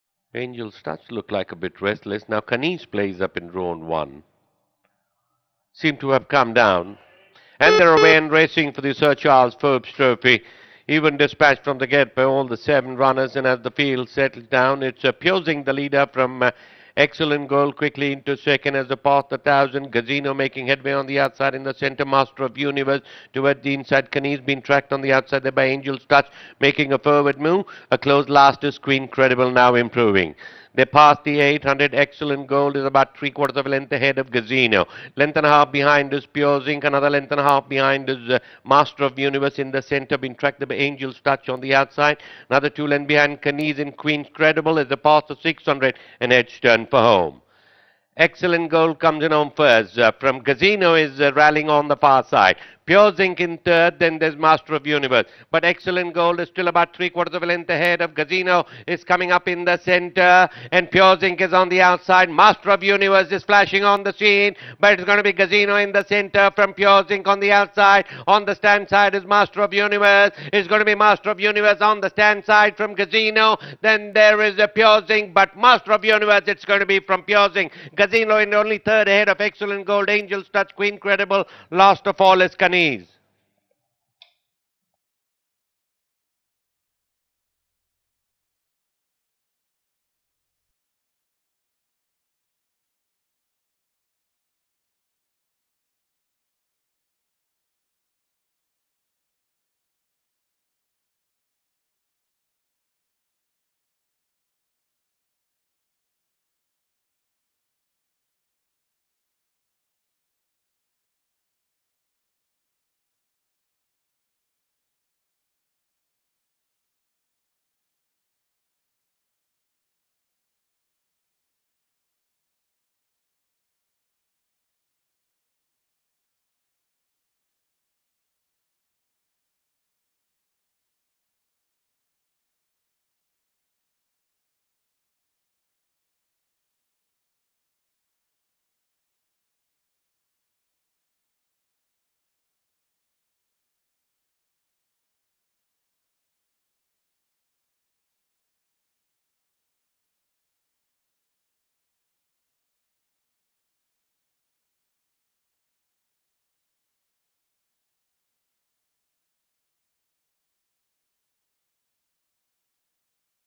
Master Of Universe with Nirmal Jodha up wins The Sir Charles Forbes Trophy 2019
Master Of Universe trained by Bipin Salvi with Nirmal Jodha up wins The Sir Charles Forbes Trophy 2019 at The Royal Western India Turf Club Mumbai